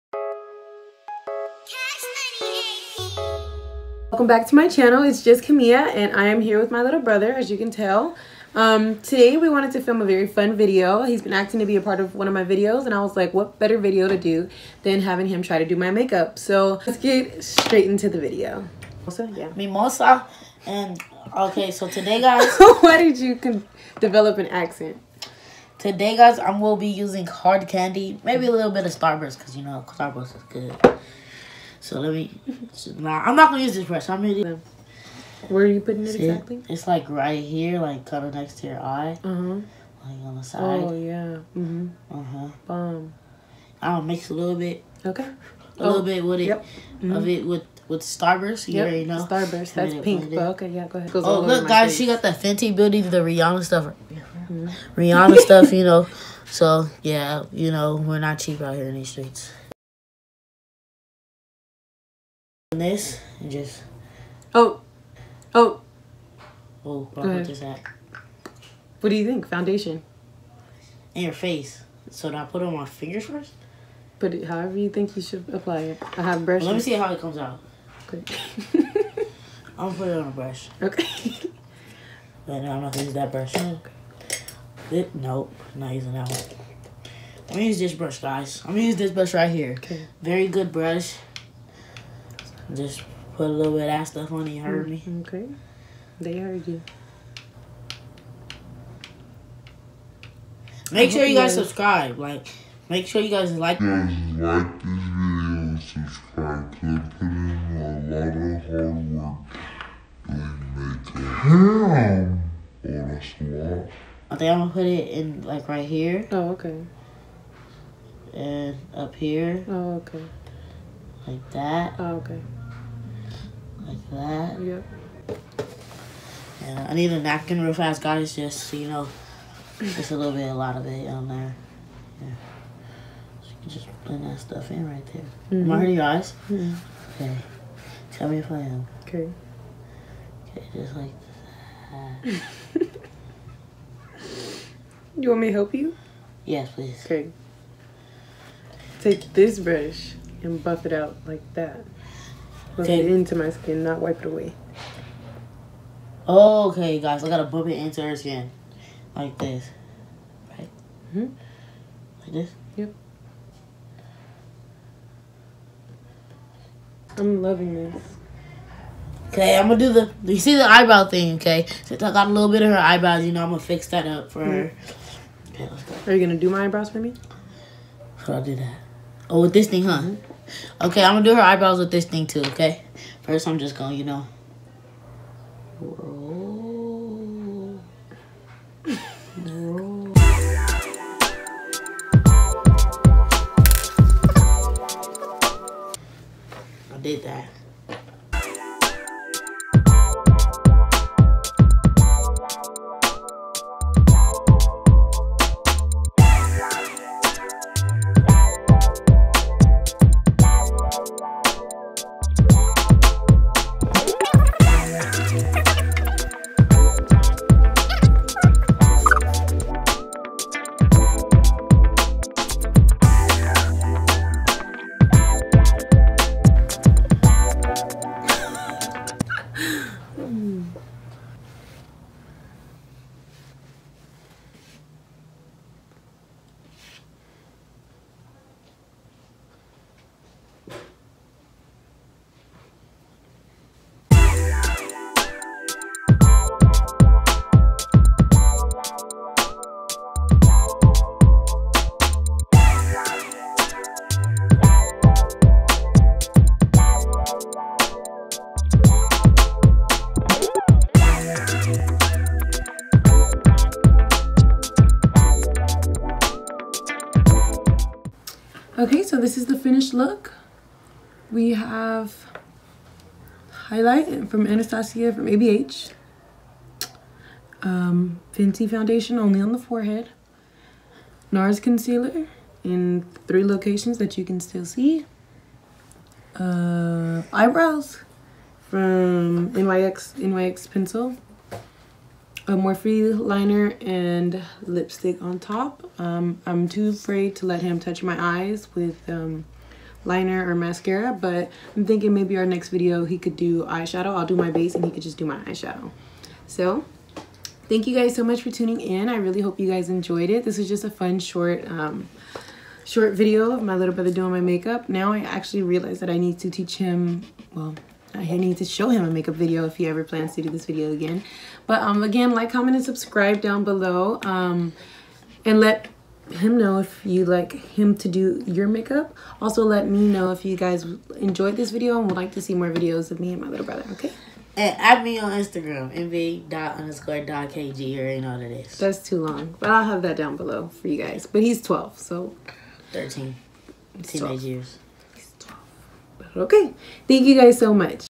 HE DOES MY MAKE-UP FOR THE FIRST TIME
Hey guys! Thank you so much for tuning into my little brother doing my make-up today. He is definitely below beginner level, so I hope you guys got a great laugh! Below is all the details for the make up used. Stay Blessed (:

Make-Up Used:
Lip Stick: